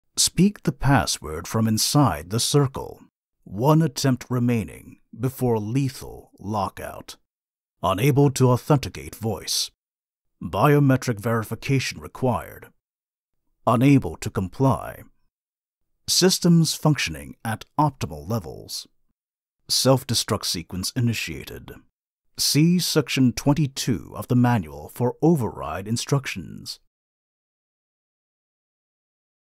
Warp Frontier Character Spotlight - Edwyn Tiong as various Computers.
Edwyn Tiong plays various nefarious computers. Edwyn didn't fit any of the existing roles, but I was glad to find a part, even though small, for his unique voice and delivery.